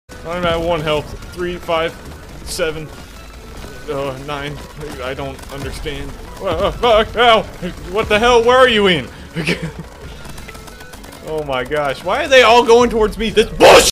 KEEP FIGHTING CRAB
I am out here with my crab fighting for my life while my friend is off doing everything besides helping me